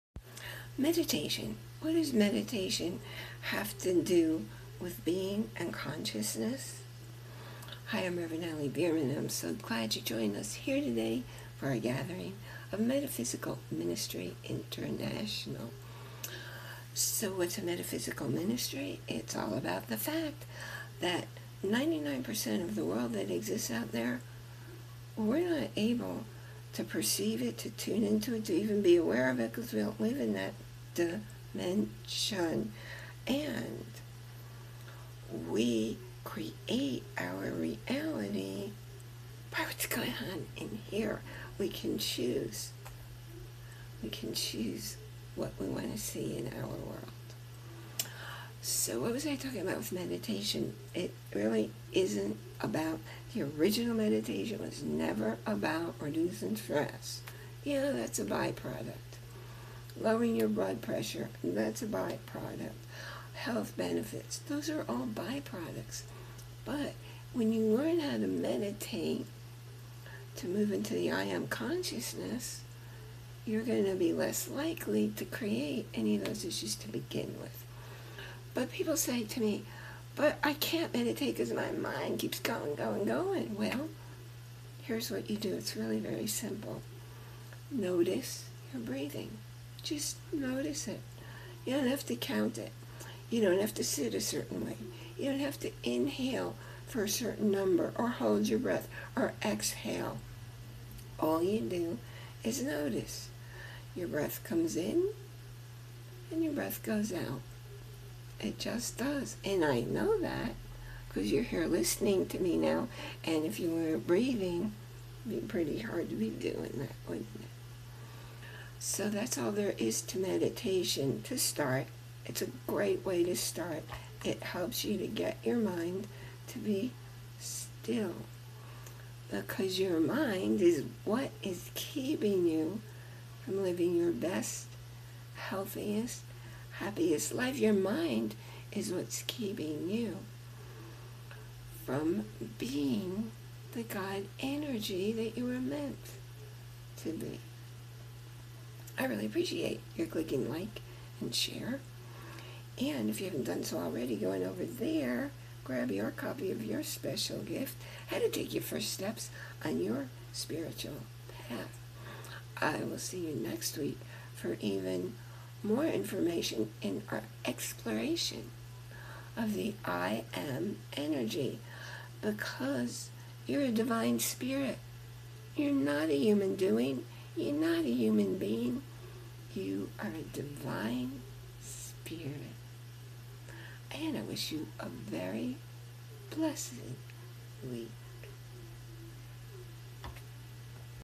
Why and How to Meditate
Meditation is not about reducing stress or assigning affirmations. those are by-products. 'here is what meditation does